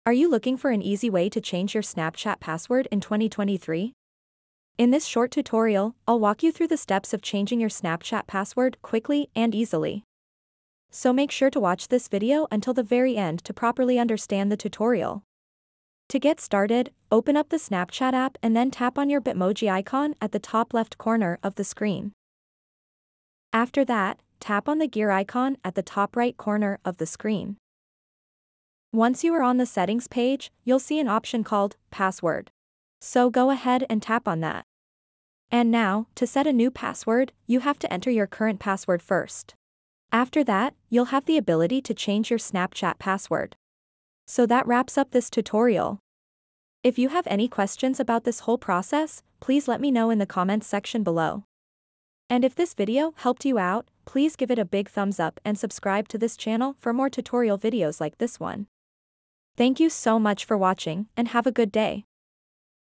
How To Change Your Snapchat Password 2023
Are you worried about your Snapchat security and want to know how to change your password in 2023? Look no further! This video will provide step-by-step instructions on how to easily update your Snapchat password. We'll show you the simple process of accessing the settings menu, navigating through the account options, and changing your current password for a stronger one. Don't miss out on this helpful guide - watch now for an easy way to keep your account secure!